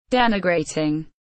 How to pronounce Denigrating / Denigrating pronunciation
Raccoon teaches you how to pronounce Denigrating . Raccoon vous apprends a prononcer Denigrating . Como pronunciar Denigrating